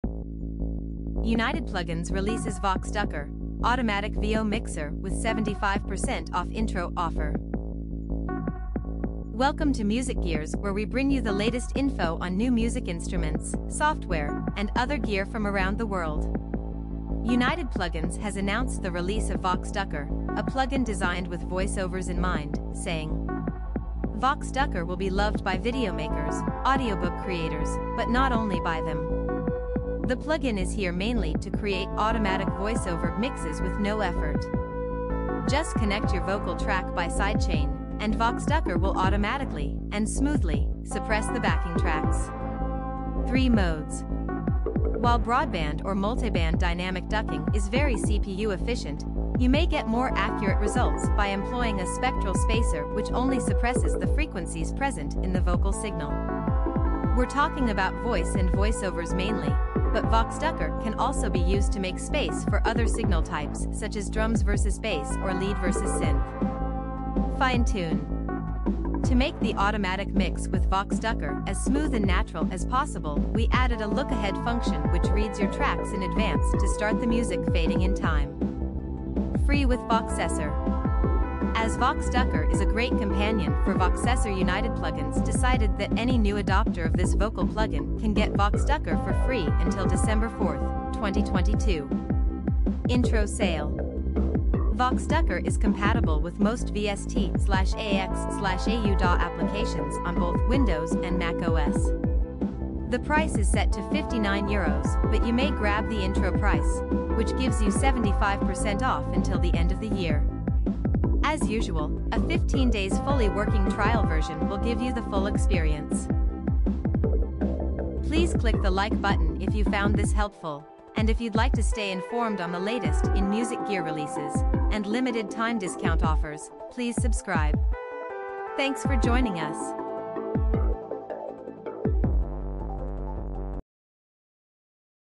UnitedPlugins releases VoxDucker: Automatic VO Mixer with 75% off intro offer
UnitedPlugins has announced the release of VoxDucker, a plugin designed with voice-overs in mind, saying:

VoxDucker will be loved by video-makers, audiobook creators, but not only by them. The plugin is here mainly to create automatic voice-over mixes with no effort. Just connect your vocal track by side-chain, and VoxDucker will automatically (and smoothly) suppress the backing tracks.

Three modes
While broad-band or multi-band dynamic ducking is very CPU efficient, you may get more accurate results by employing a spectral spacer which only suppresses the frequencies present in the vocal signal. We're talking about voice and voice-overs mainly, but VoxDucker can also be used to make space for other signal types - such as drums vs bass or lead vs synth.

Fine-tune
To make the automatic mix with VoxDucker as smooth and natural as possible - we added a look-ahead function which reads your tracks in advance to start the music fading in time.

Free with Voxessor
As VoxDucker is a great companion for Voxessor UnitedPlugins decided that any new adopter of this vocal plugin can get VoxDucker for free until December 4, 2022.

Intro Sale
VoxDucker is compatible with most VST/AAX/AU DAW applications on both Windows and macOS. The price is set to €59 but you may grab the intro price, which gives you 75% off until the end of the year. As usual, a 15-days fully working trial version will give you the full experience.